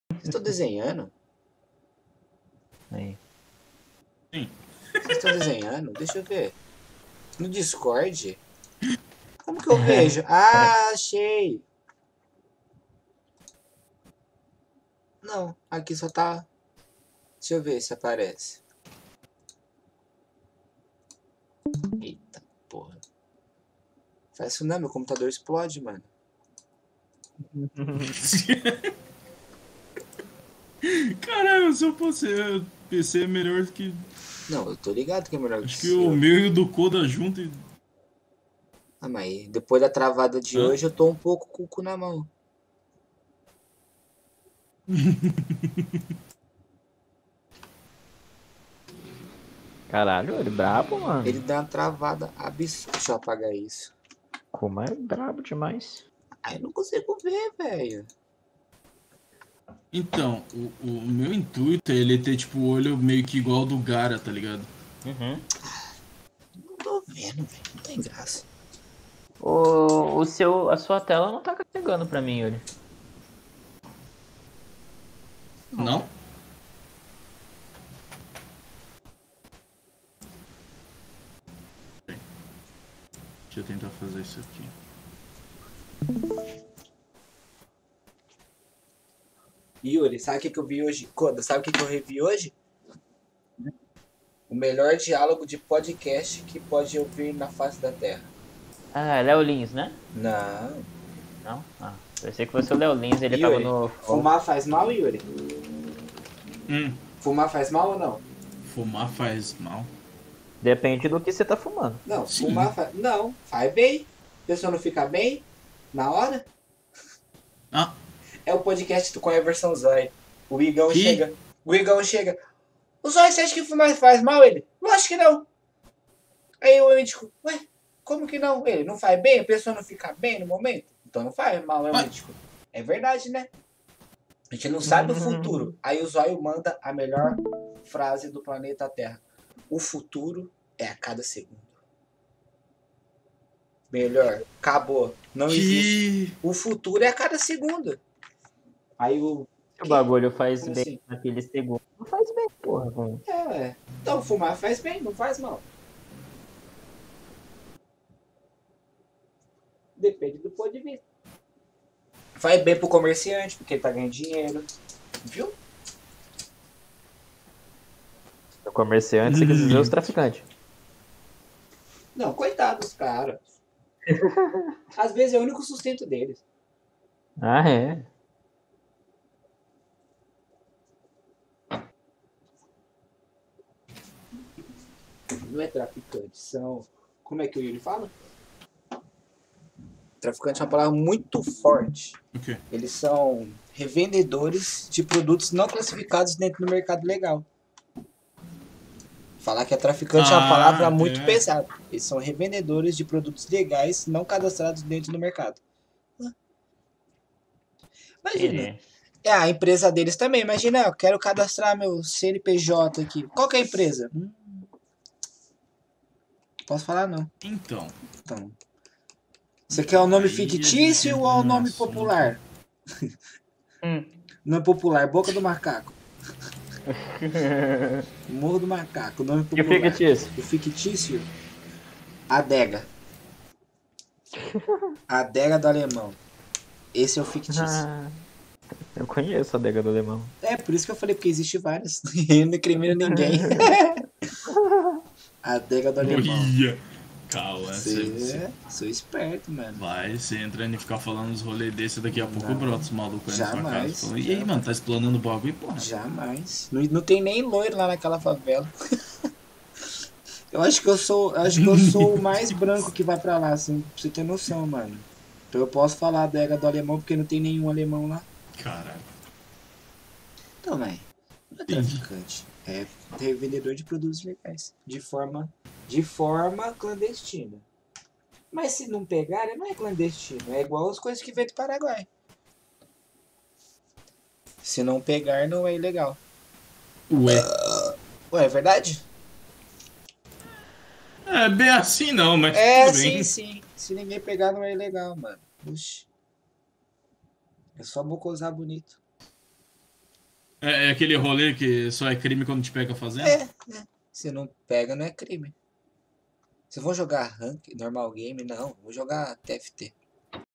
KAUAN N SABE USAR O DISCORD, TRAFICANTE = REVENDEDORES DE PRODUTOS N CLASSIFICADOS NO MERCADO LEGAL
BEER AND WEED PODCAST EDIÇÃO 42
 KKKKKKKKKKK EU NÃO SEI USAR O DISCORD PRA VER LIVE KKKK
E CLASSIFICANDO COMO SERIA "TRAFICANTE" NO MERCADO DE TRABALHO
E HJ TEM O KODAAAA JUNTO PRA ESSA CONVERSA KKK